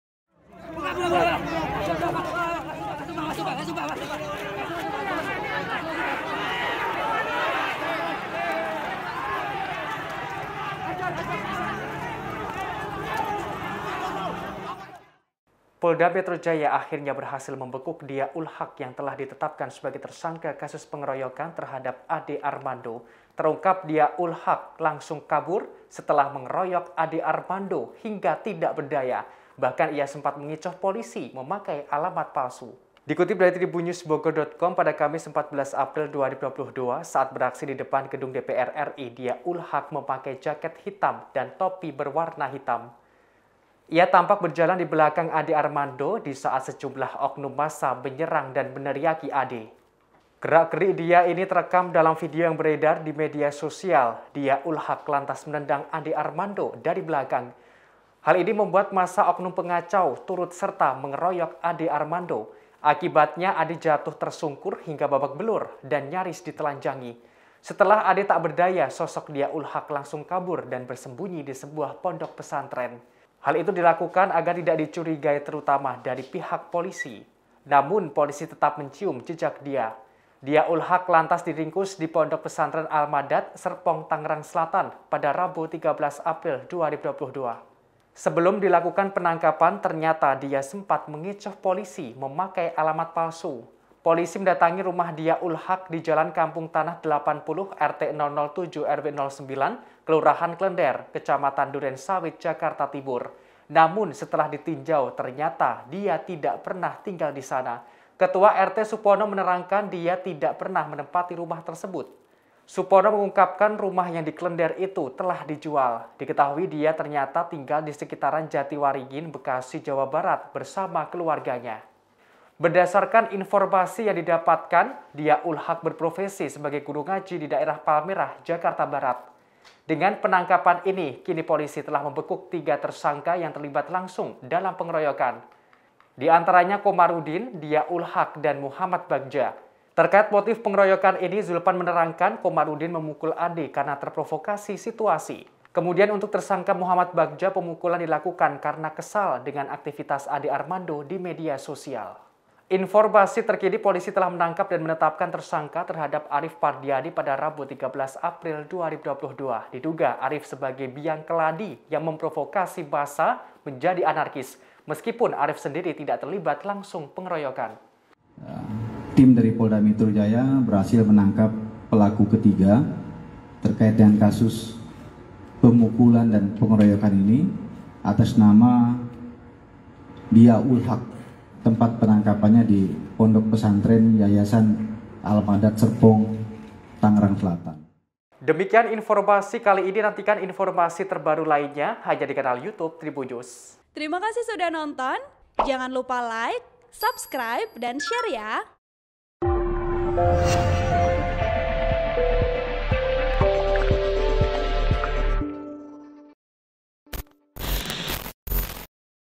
Dhia Ul Haq Langsung Kabur seusai Keroyok Ade Armando, Sempat Kecoh Polisi dengan Alamat Palsu
TRIBUN-VIDEO.COM - Polda Metro Jaya akhirnya berhasil membekuk Dhia Ul Haq yang telah ditetapkan sebagai tersangka kasus pengeroyokan terhadap Ade Armando.

Terungkap, Dhia Ul Haq langsung kabur setelah mengeroyok Ade Armando hingga tak berdaya.

Bahkan, ia sempat mengecoh polisi memakai alamat paslu.

Dikutip dari TribunnewsBogor.com pada Kamis (14/4/2022), saat beraksi di depan Gedung DPR RI, Dhia Ul Haq memakai jaket hitam dan topi warna hitam.

Ia tampak berjalan di belakang Ade Armano disaat sejumlah oknum massa menyerang dan meneriaki Ade.

Dhia Ul Haq lantas menendang Ade Armando dari belakang.

Hal ini membuat massa oknum pengacau turut serta mengeroyok Ade Armando.

Akibatnya, Ade jatuh tersungkur hingga babak belu rdan nyaris ditelanjangi.

Setelah Ade tak berdaya, sosok Dhia Ul Haq langsung kabur dan bersembunyi di sebuah pondok pesantren.

Hal itu dilakukan agar tidak dicurigai terutama dari pihak polisi.

Namun, polisi tetap mencium jejak Dhia.

Dhia Ul Haq lantas diringkus di Pondok Pesantren Al Madad, Serpong, Tangerang Selatan pada Rabu (13/4/2022).

"Tepatnya dini hari tadi, pukul 02.30 WIB, tim dari Polda Metro Jaya berhasil menangkap pelaku ketiga, terkait dengan kasus pemukulan Ade Armando, atas nama Dhia Ul Haq."

Yang bersangkutan kita tangkap di lokasi di Pondok Pesantren Al Madad Serpong Tangerang Selatan," ungkap Ksbid Humas Polda Metro Jaya, Kombes Pol Endra Zulpan.

Sebelum dilakukan penangkapan, ternyata Dhia smepat mengecoh polisi memakai alamat palsu.

Polisi mendatangi rumah Dhia Ul Haq di Jalan Kampung Tanah 80, RT 007 RW 09, Kelurahan Klender, Kecamatan Duren Sawit, Jakarta Timur.

Namun setelah ditinjau, ternyata Dhia tidak pernah tinggal di sana.

Ketua RT, Supono menerangkan, Dhia tidka pernah menempati rumah tersebut.

"Alamatnya benar di sini, tapi nggak pernah ada tinggal di sini,” ungkap Supono, di lokasi, dikutip TribunnewsBogor.com dari Tribunnews, Senin (11/4/2022).

Supono mengungkapkan, rumah yang di Klender itu telah dijual.

Diketahui, Dhia ternyata tinggal di sekitaran Jartiwaringin Bekasi, Jawa Barat, bersama keluarganya.

"Setelah rumah ini dijual, dia pindah ke sana. Ngontrak di daerah Jatiwaringin." ujar Supono

Berdasarkan informasi yang didapatkan, Dhia Ul Haq berprofesi sebagai guru ngaji di daerah Palmerah, Jakarta Barat.

Dengan penangkapan ini, kini polisi telah membekuk tiga tersangka yang terlibat langsung dalam pengeroyokan.

Di antaranya Komarudin, Dhia Ul Haq, dan Muhammad Bagja.

Terkait motif pengeroyokan ini, Zulpan menerangkan Komarudin memukul Ade karena terprovoksi situasi.

"Komarudin melakukan pemukulan karena terprovokasi dengan situasi di TKP. Dia lantas ikut untuk menghajar korban saat demo di DPR," ujar Zulpan.

Infromasi terkini, polisi telah menangkap dan menetapkan tersangka terhadap Arif Pardiani pada Rabu (13/4/2022).

Diduga Arif sebagai biang keladi yang memprovokasi massa menjadi anarkis.

Meskipun Arif sendiri tak terlibat langsung pengeroyokan.

(Tribun-Video.com/ TribunnewsBogor.com)

Artikel ini telah tayang di TribunnewsBogor.com dengan judul Kabur ke Pesantren Usai Keroyok Ade Armando, Dhia Ul Haq Sempat Kecoh Polisi Pakai 'Alamat Palsu',
Penulis: Uyun | Editor: Damanhuri

Video Editor : Ghozi Luthfi R